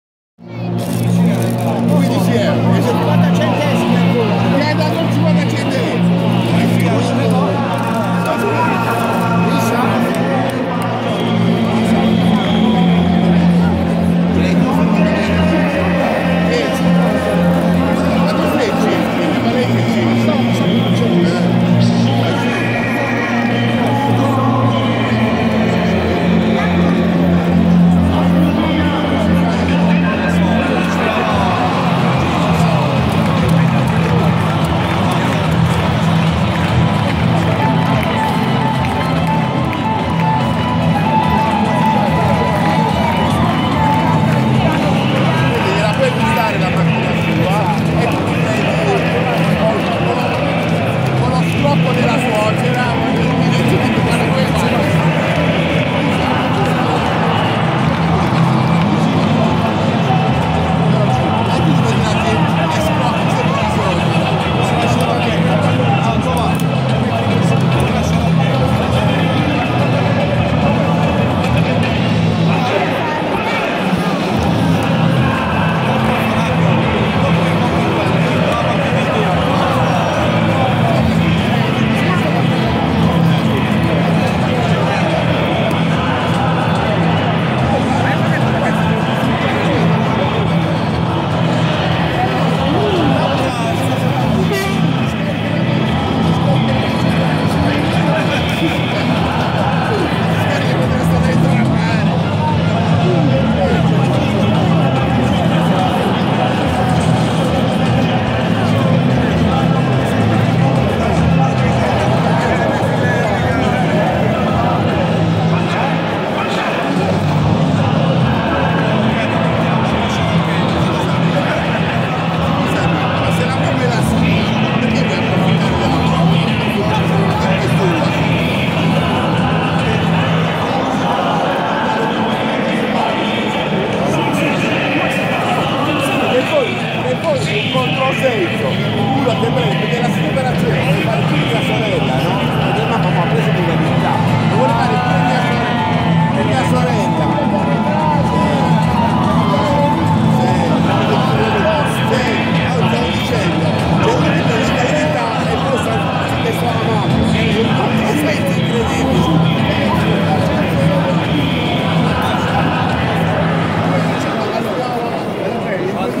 Bari - Livorno 1-0. Prepartita con musica di Gianni Ciardo
Bari, 22/11/2015
Stadio San Nicola
Curva Sud anello superiore
Campionato di calcio di Serie B 2015/2016